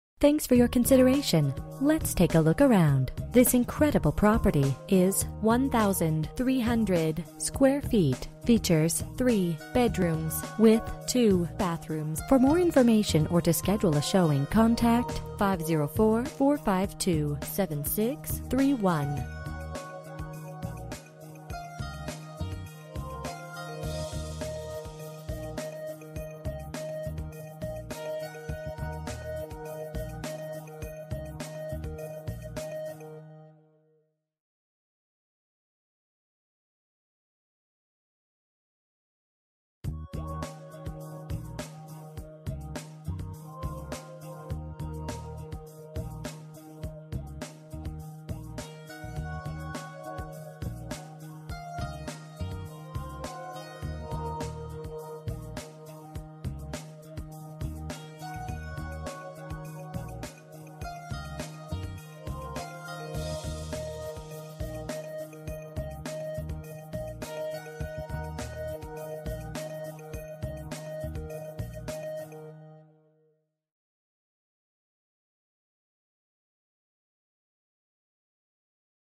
2411 MALLARD Street for sale in Slidell, LA 70460 - Property
Why rent when you can own?  Take a look at this recently renovated 3 bedroom and 2 bath home.  Home located on oversized Lot and conveniently located minutes from the all the action.  Relax on the covered patio made for entertaining.  Renovations include New kitchen with Stainless Steel Appliances and granite counter-tops, fresh paint and new flooring.  Home qualifies for 100% RD financing!  Flood Zone X!  Come see today!

Bedrooms: 3
Bathrooms: 2
Square Feet: 1300
Price: $154,900